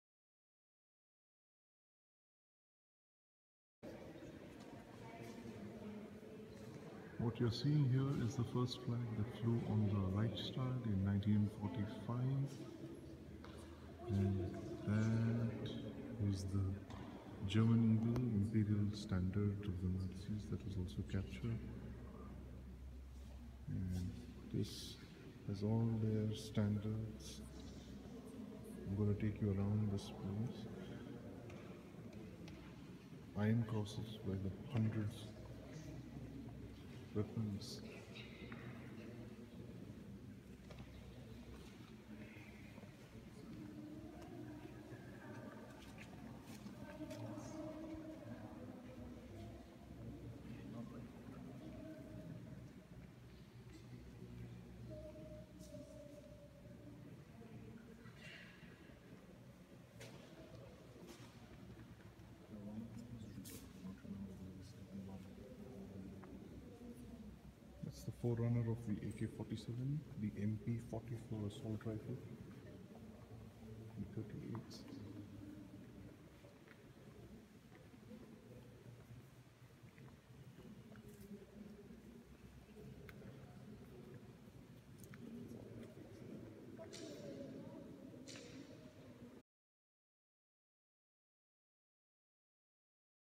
The defeat of Nazi Germany -- Moscow central military museum.
The iconic flag hoisted atop the Reichstag on May 2, 1945 and shot by Yevgeny Khaldei now sits atop a scintillating exhibit on the defeat of Nazi Germany at Moscow's Central Military Museum. The museum has the largest collection of captured weapons I've seen (gun enthusiasts note the StG- 44, believed to have inspired the AK-47, at 1:08) and indeed of Iron Crosses taken from the defeated German forces during the Battle of Berlin.